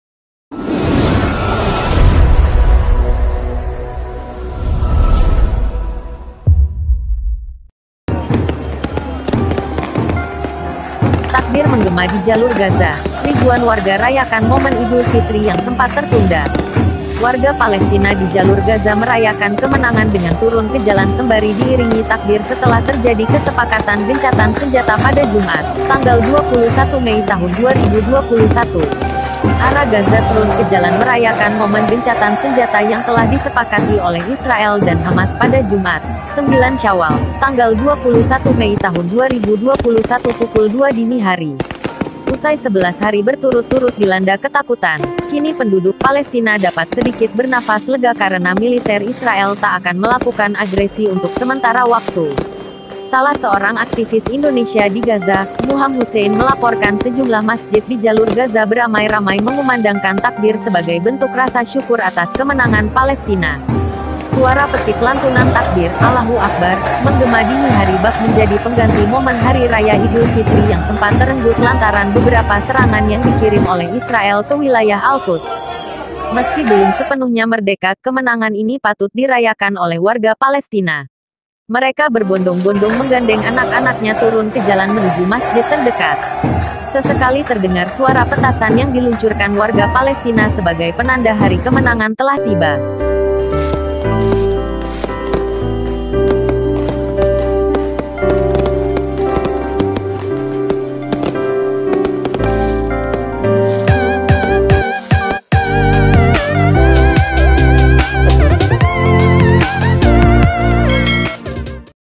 inilah momen haru Palestina usai peperangan dengan Israel
momen haru Palestina usai berakhirnya peperangan humas Palestina dengan Israel..